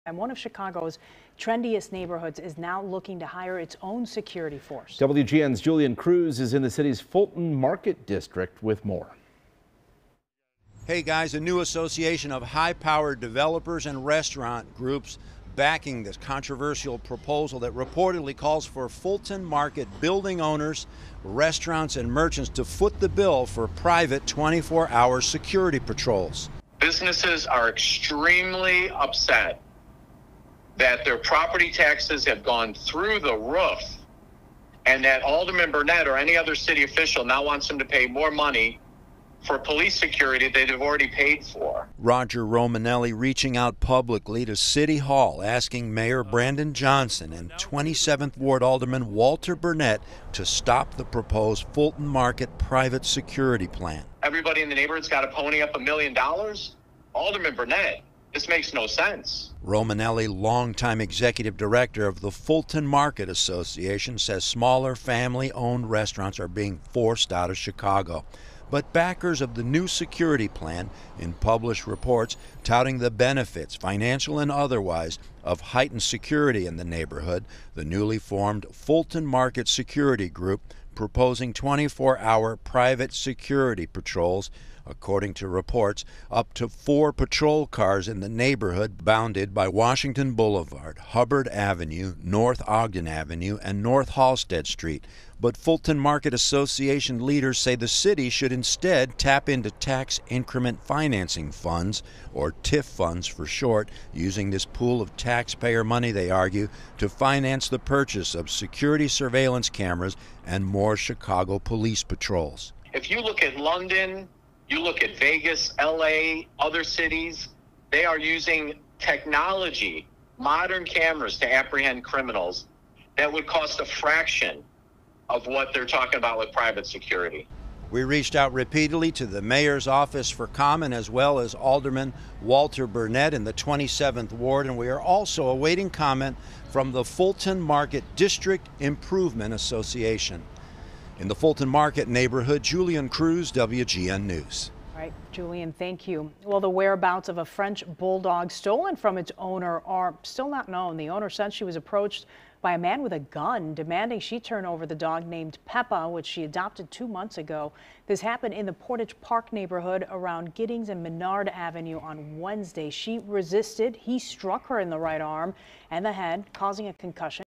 Fulton Market developers, restaurant groups look to hire private 24 hour security
A proposal to hire private security to patrol Fulton Market has been drawing fire Friday afternoon.